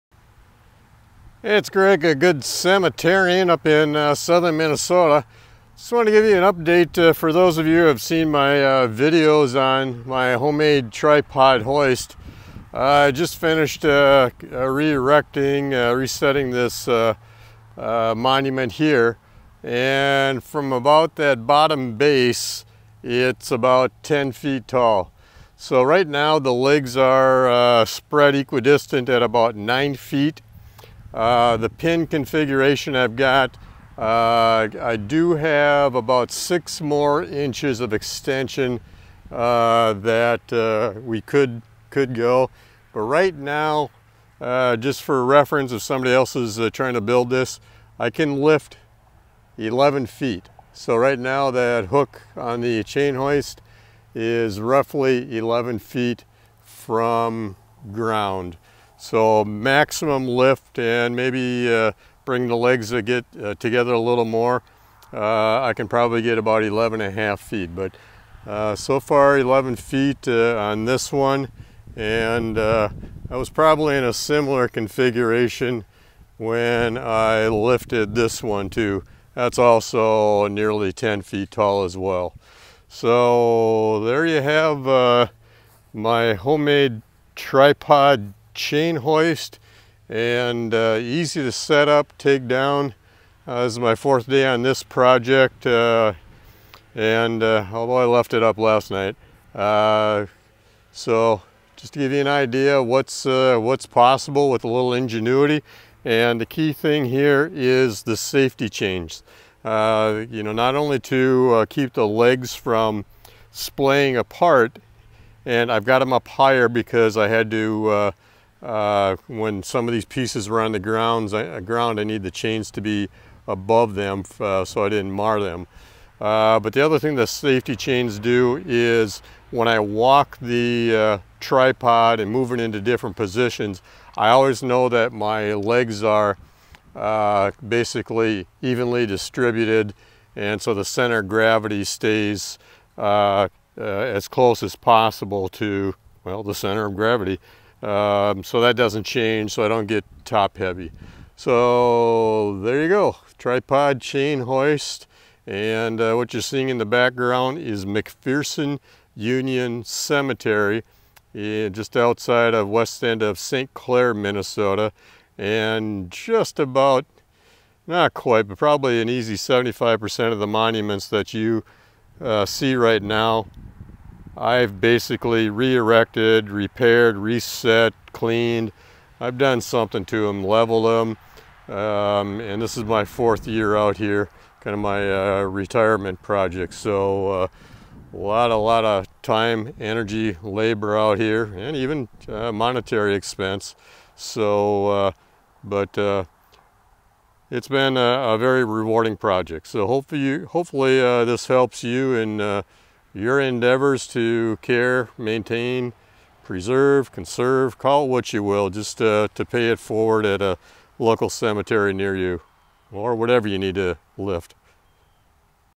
Tripod chain hoist used in cemetery restoration
DIY Tripod chain hoist that I made three years ago, in use during cemetery restoration. Has over 11 feet of lift